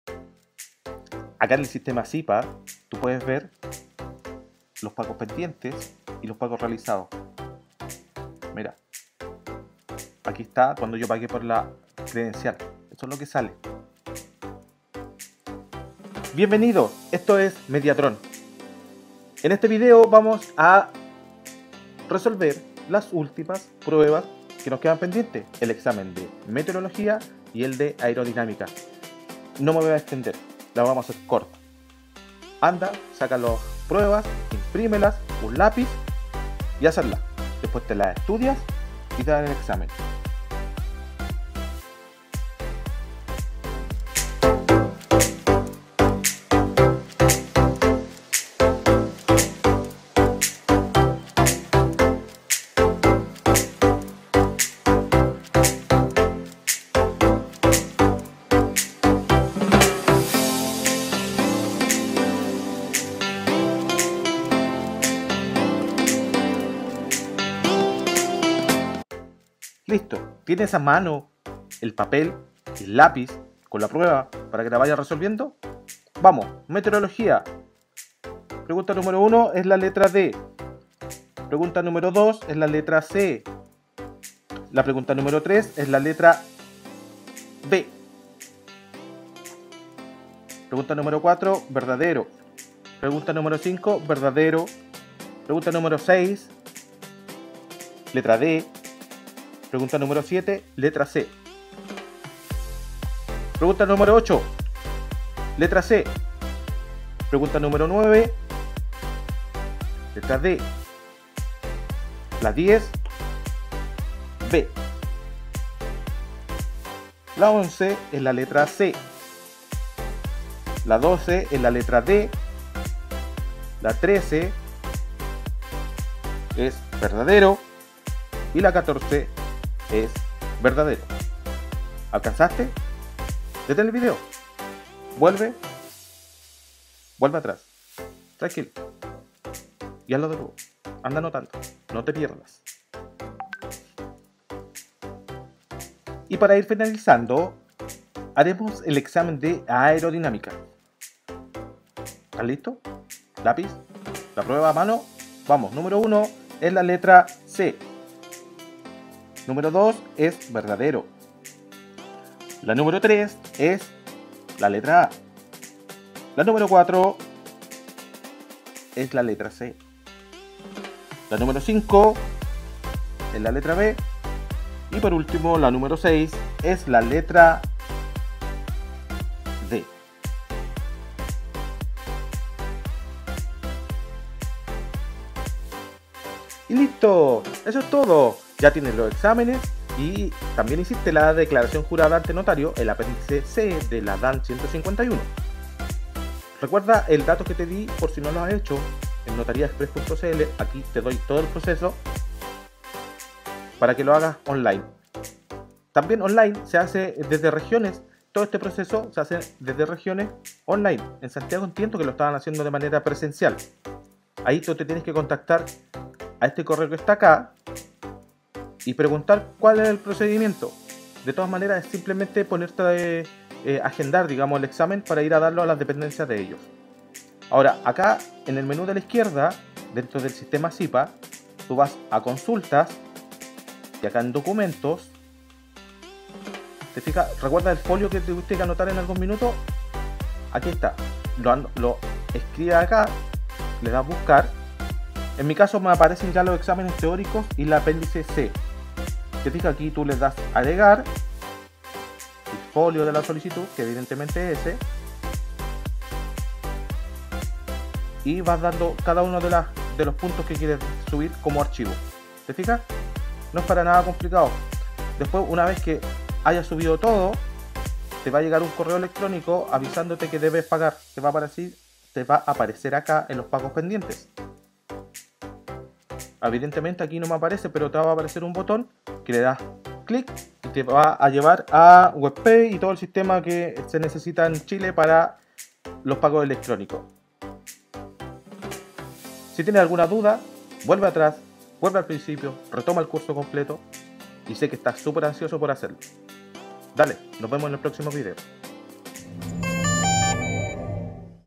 [RESPUESTAS] Examen Meteorología y Aerodinámica 📒 Desarrollo 🚁 🎮 Curso [GRATIS] Piloto de Drone
👉  aqui tienes la solución a este examen, con éste ya tendras todo para postular y obtener tu credencial de la DGAC.